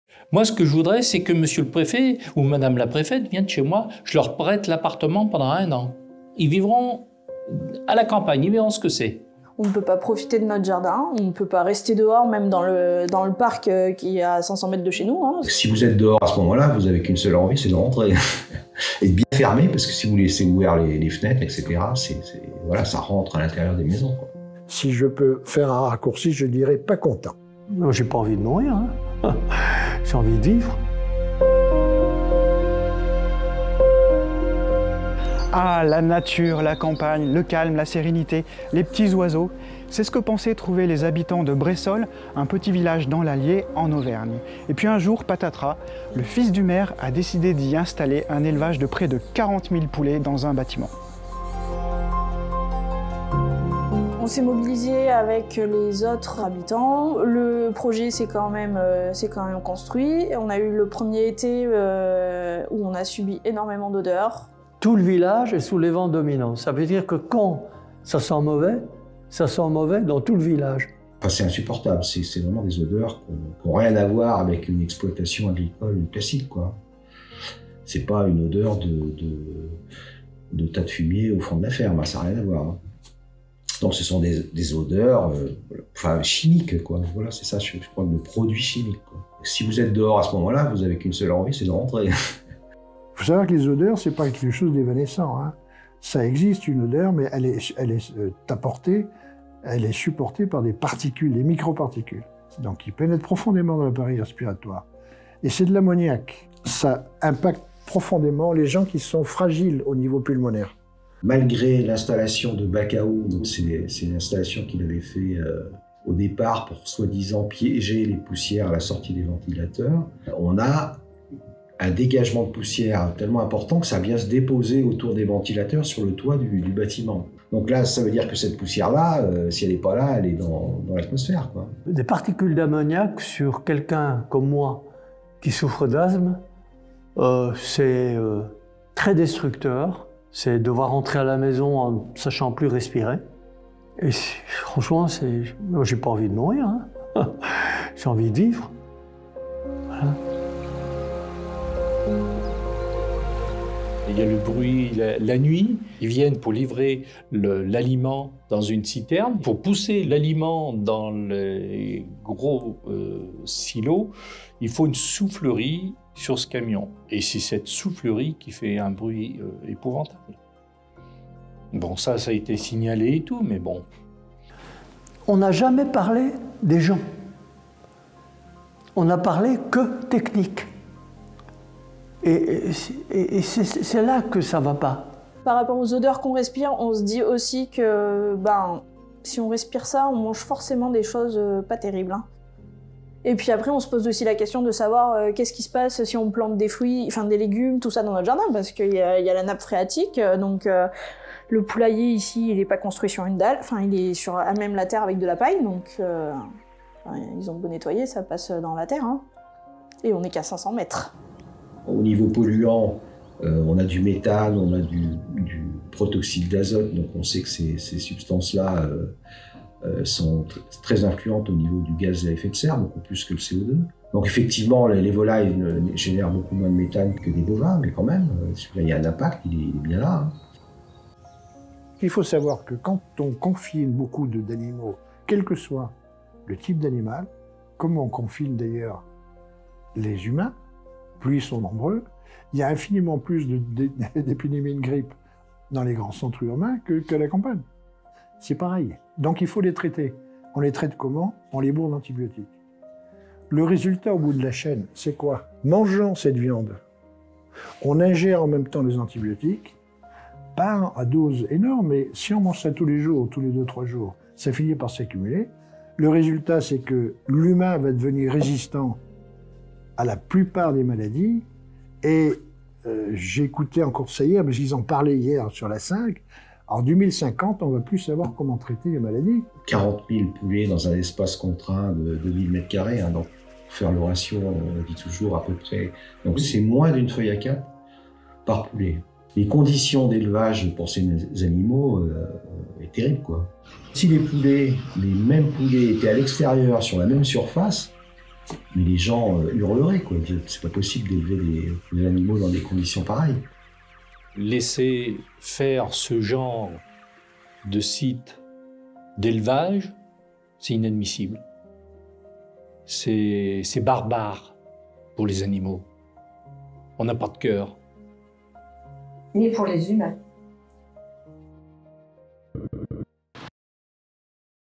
Vivre à côté d’un élevage intensif : des témoignages poignants
Les habitants de Bressolles (Allier) vivent actuellement à côté d’un élevage intensif de 30 000 poulets.
Malgré les manifestations, les plaintes, les nombreuses alertes auprès du maire et du préfet, les riverains subissent de multiples nuisances, odeurs, bruits, pollution. Ils témoignent aujourd’hui de leur quotidien.
L’association de riveraines et de riverains, Bressolles Bien Vivre, L214 et France Nature Environnement 03 avaient obtenu l’annulation du projet initial qui concernait 40 000 poulets. L’éleveur avait fait appel et avait obtenu gain de cause. Les associations ont saisi le Conseil d’Etat.
Le dossier est encore entre les mains de la justice.

Se mobiliser contre les projets d’installation ou d’extension d’élevages intensifs est une nécessité, pour les animaux mais aussi pour les humains et l’environnement.
Signez la pétition.